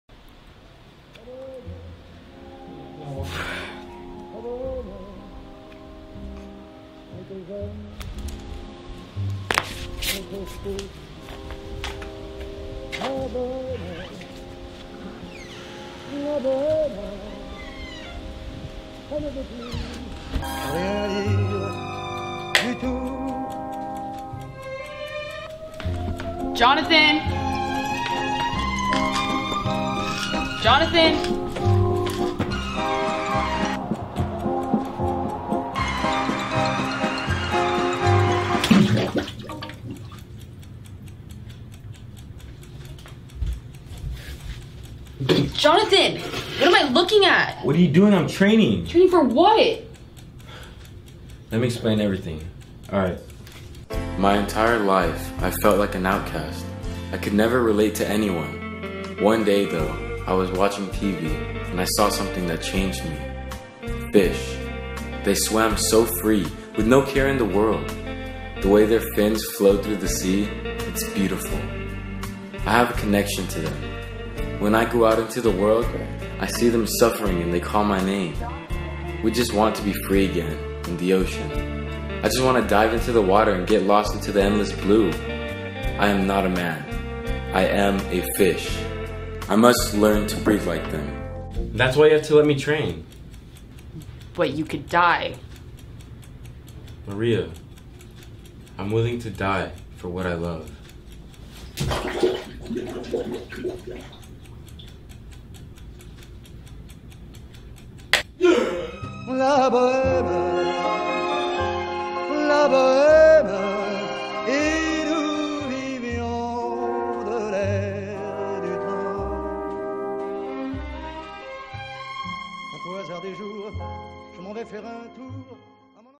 The Birth of a Fish
Made for the DCFI 48 Hour Film Race

dictated by me, Joseph F. Treviño, and Jonathan Orendain
starring Jonathan Orendain and Maria Gamba